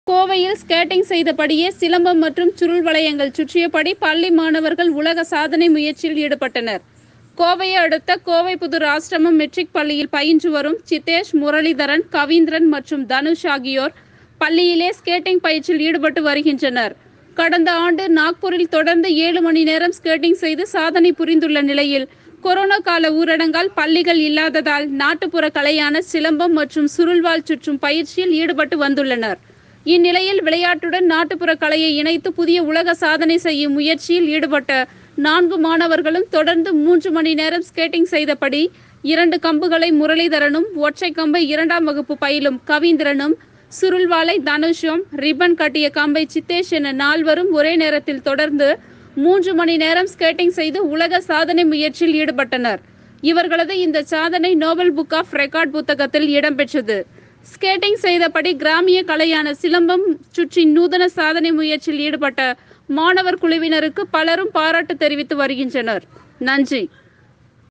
கோவையில் ஸ்கேட்டிங் செய்தபடியே சிலம்பம் மற்றும் சுருள் வளையங்கள் சுற்றிபடி பள்ளி மாணவர்கள் உலக சாதனை
கோவையில் ஸ்கேட்டிங் செய்தபடியே சிலம்பம் மற்றும் சுருள் வளையங்கள் சுற்றிபடி பள்ளி மாணவர்கள் உலக சாதனை முயற்சியில் ஈடுபட்டனர்…

கோவையை அடுத்த கோவைப்புதூர் ஆஸ்ரமம் மெட்ரிக் பள்ளியில்  பயின்று வரும் சித்தேஷ்,முரளிதரன்,கவிந்திரன் மற்றும் தனுஷ் ஆகியோர் பள்ளியிலே ஸ்கேட்டிங் பயிற்சியில் ஈடுபட்டு வருகின்றனர்.  கடந்த ஆண்டு நாக்பூரில் தொடர்ந்து ஏழு மணி நேரம் ஸ்கேட்டிங் செய்து சாதனை புரிந்துள்ள, நிலையில் கொரோனா கால ஊரடங்கால் பள்ளிகள் இல்லாததால்   நாட்டுப்புற கலையான சிலம்பம் மற்றும் சுருள்  வாள் சுற்றும் பயிற்சியில் ஈடுபட்டு வந்துள்ளனர்.  இந்நிலையில் விளையாட்டுடன் நாட்டுபுற கலையை இணைத்து  புதிய உலக சாதனை செய்யும் முயற்சியில் ஈடுபட்ட நான்கு மாணவர்களும்,.தொடர்ந்து மூன்று மணி நேரம் ஸ்கேட்டிங் செய்தபடி இரண்டு கம்புகளை முரளிதரனும்,ஒற்றை கம்பை இரண்டாம் வகுப்பு பயிலும் கவிந்தரனும்,சுருள் வாளை தனுசும்,ரிப்பன் கட்டிய கம்பை சித்தேஷ் என நால்வரும் ஒரே நேரத்தில் தொடர்ந்து மூன்று மணி நேரம் ஸ்கேட்டிங் செய்து உலக சாதனை முயற்சியில் ஈடுபட்டனர்.இவர்களது இந்த சாதனை நோபள் புக் ஆப் ரெக்கார்ட் புத்தகத்தில் இடம்பெற்றது.ஸ்கேட்டிங் செய்தபடி கிராமிய கலையான சிலம்பம் சுற்றி நூதன சாதனை முயற்சியில் ஈடுபட்ட மாணவர் குழுவினருக்கு பலரும் பாராட்டு தெரிவித்து வருகின்றனர்.